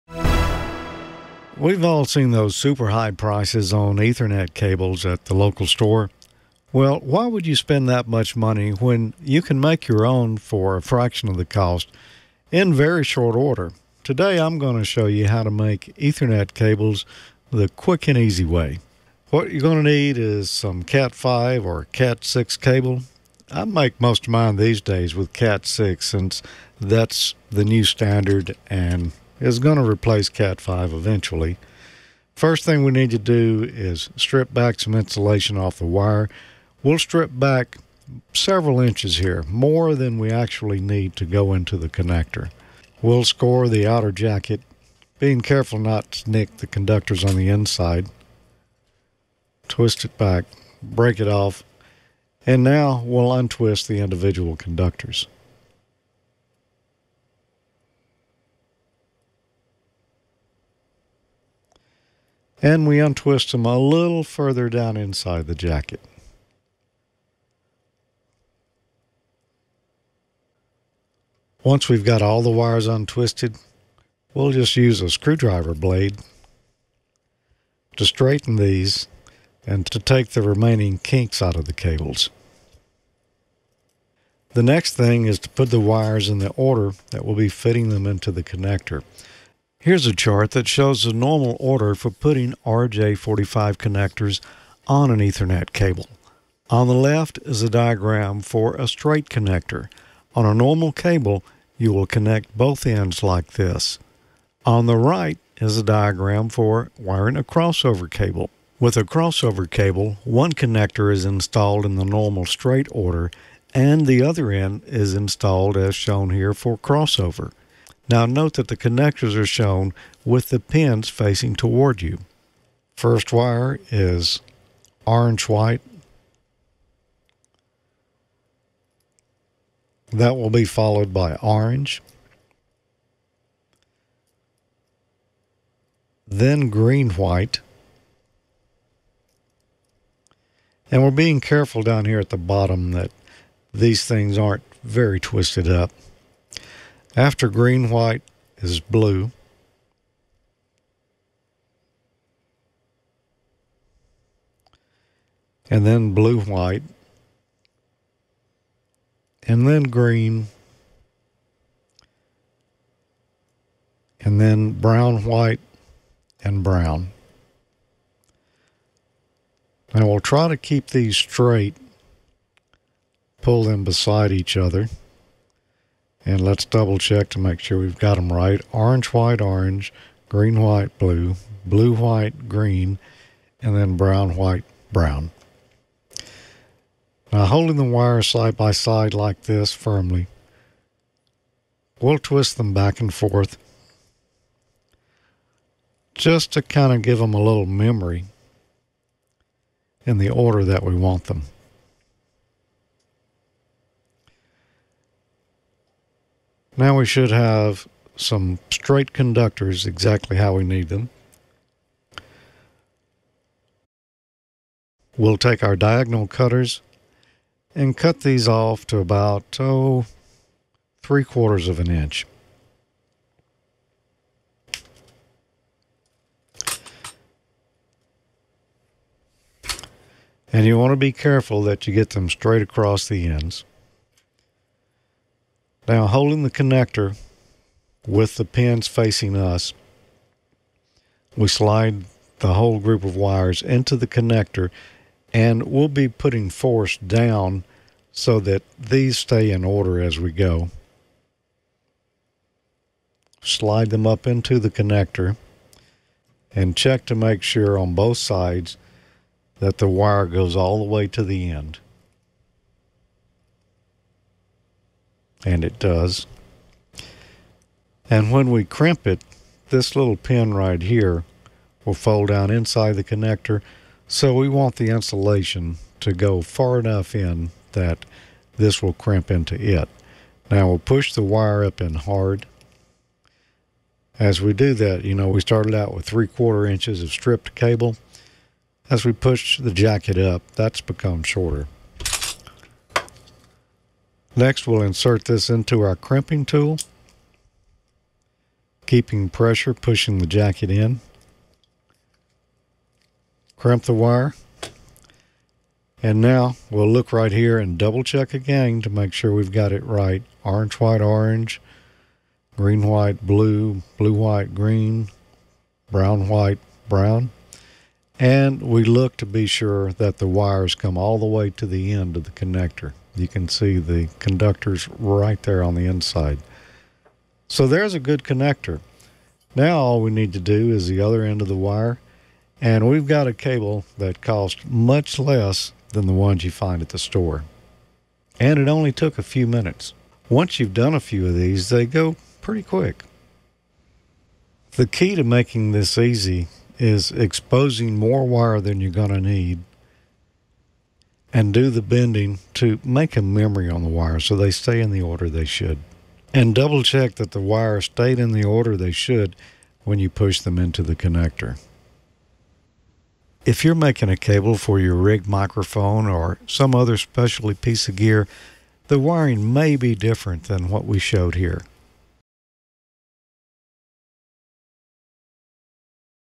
Smoke & Solder DIY RJ-45 Connector
George shows you how to make your own RJ-45 connectors for your ethernet cables!